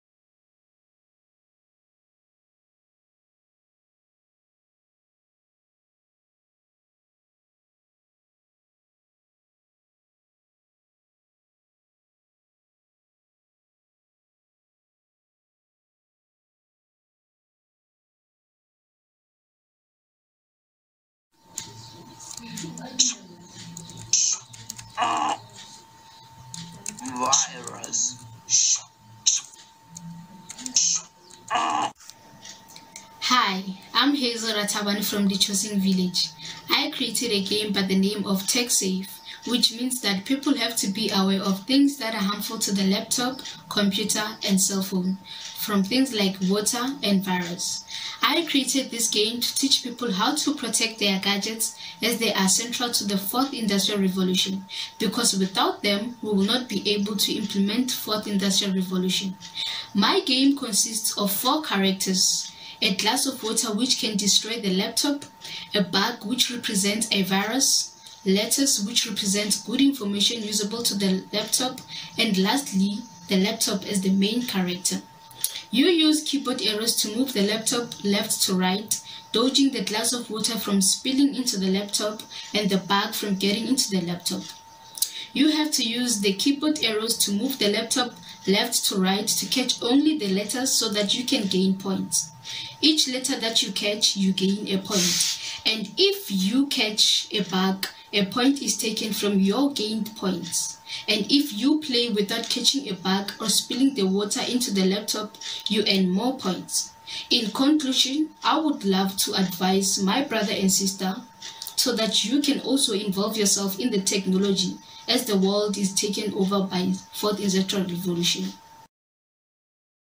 African Code Challenge 2020- Tech Safe Game (Limpopo Transformation Drivers)
African Code Challenge 2020 submission by Kelebogile Hazel Rathabane